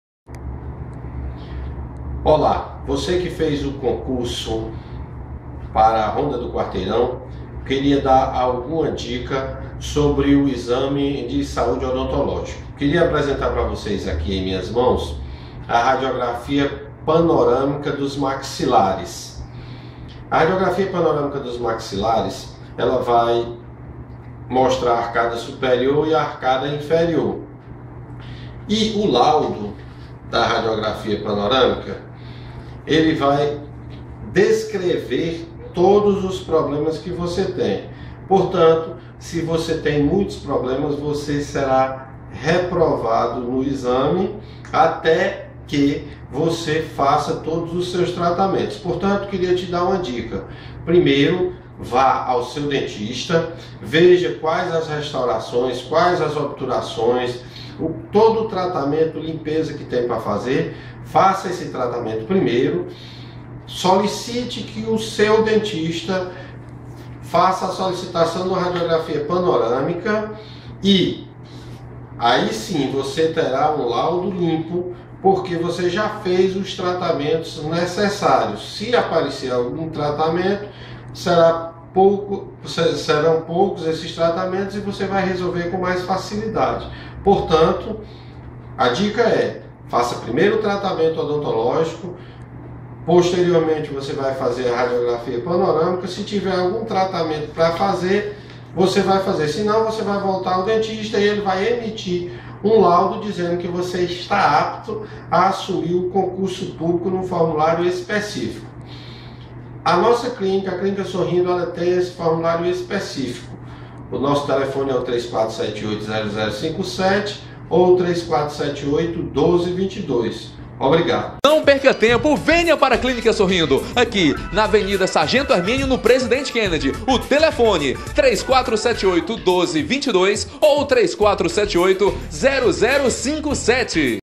inspeção de saúde concurso ronda do quarteirão 2011 2012
Ronda do Quarteirão: Divulgada lista dos aprovados em 1ª etapa de concurso

março 26, 2012

O Governo do Estado do Cearádivulgou nesta segunda-feira (26) o resultado da primeira etapa do concurso público para preenchimento de 1.000 vagas para soldado da Polícia Militar (PM-CE). O salário é de R$ 1.606,01.

O edital com o resultado da primeira etapa convoca os aprovados para a inspeção de saúde a partir do dia 20 de abril. A primeira etapa (prova objetiva) foi realizada no dia 26 de fevereiro de 2012. Para os exames médicos, os convocados devem levar radiografia panorâmica das arcadas dentárias e raios-x simples de coluna cervical, toráxica, lombar e sacra.

O concurso para soldado da Polícia Militar exige ensino médio e constará de três etapas: uma prova objetiva de caráter eliminatório e classificatório; exames médicos e teste físico de caráter eliminatório, além de um curso de formação profissional de 1.020 horas/aula com caráter eliminatório e classificatório.

Os candidatos podem obter mais informações também por meio da Central de Atendimento do Cespe/UnB, de segunda a sexta-feira, das 8 horas às 19 horas pelo telefone .